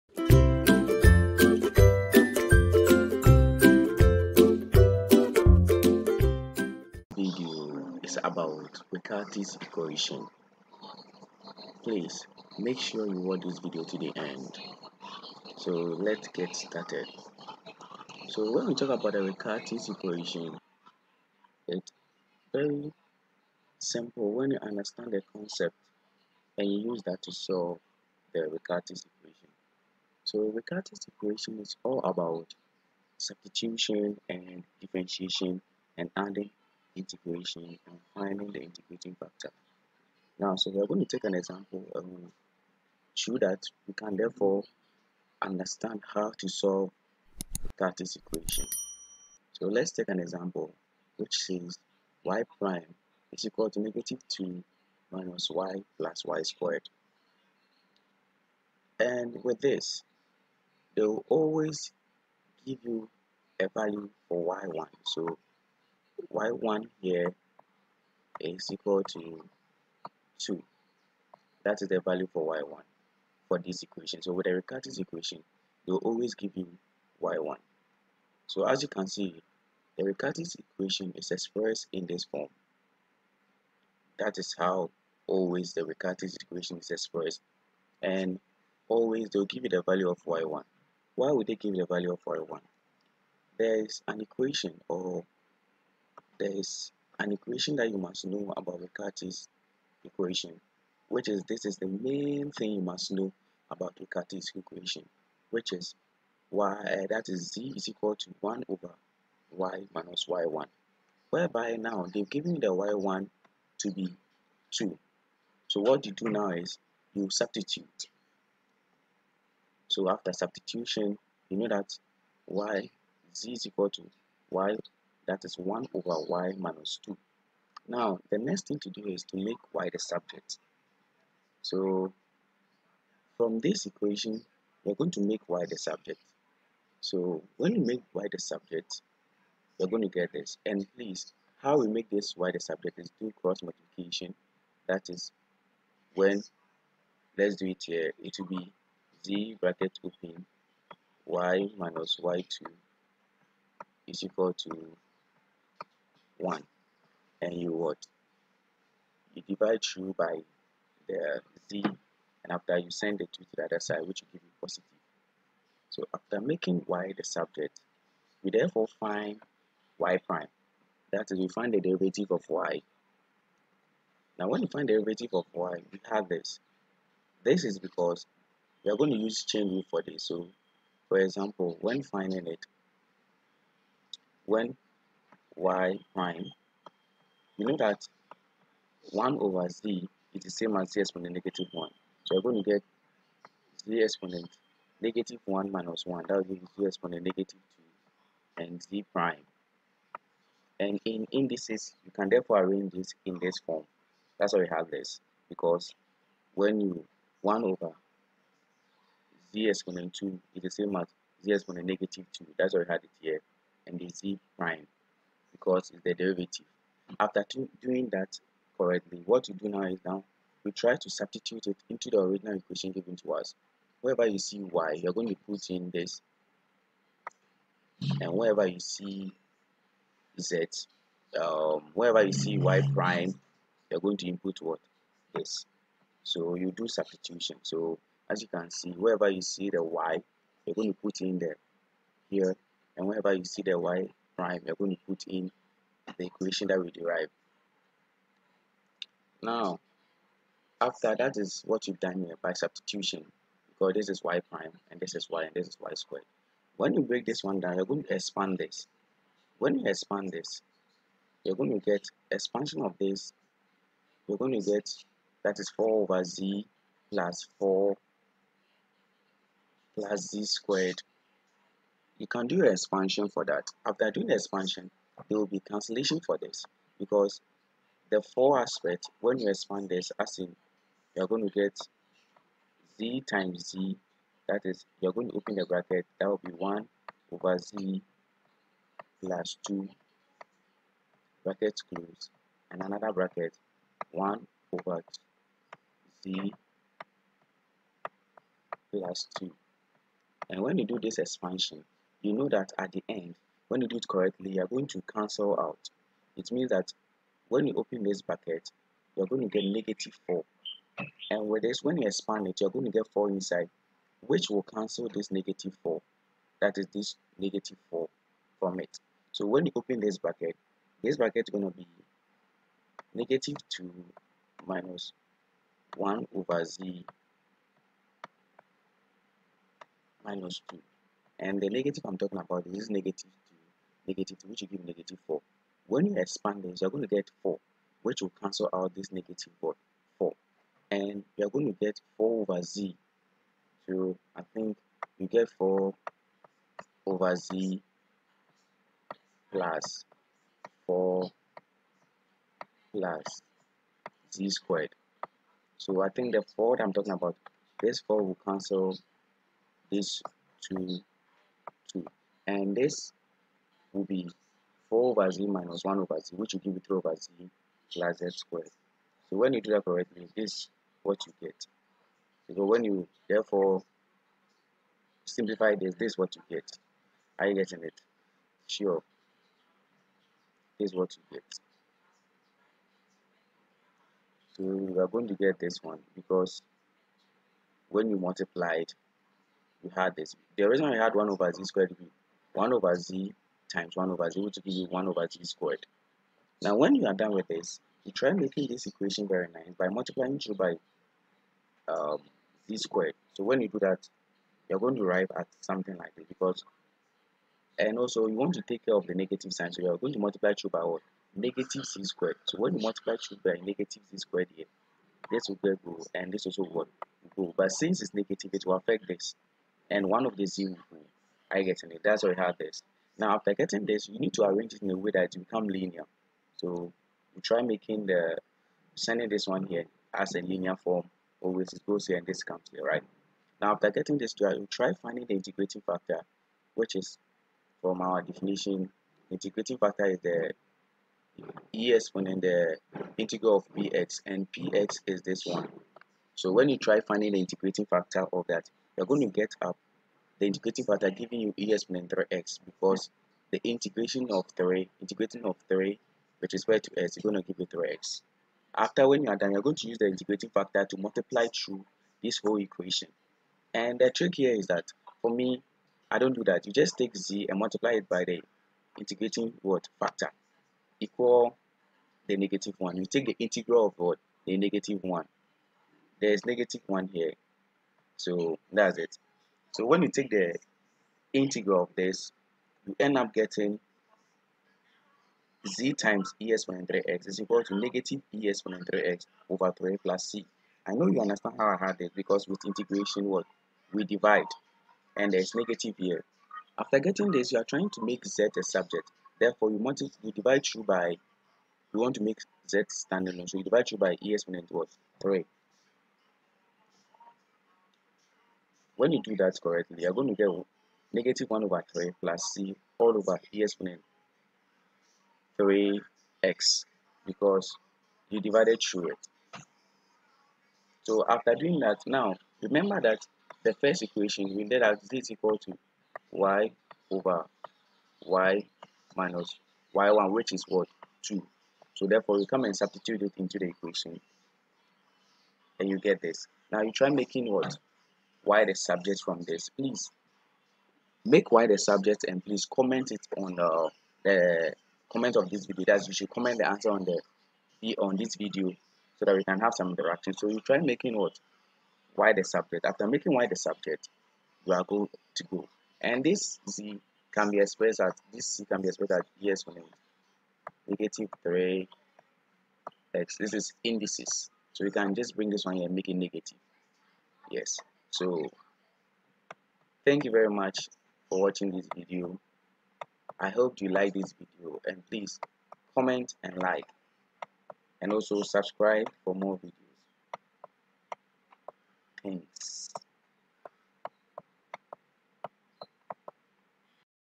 🔴 11 - DIFFERENTIAL EQUATION - Riccati Differential Equation
Riccati's equation is a topic in differential equation . This video will show you step by step on how to solve differential equation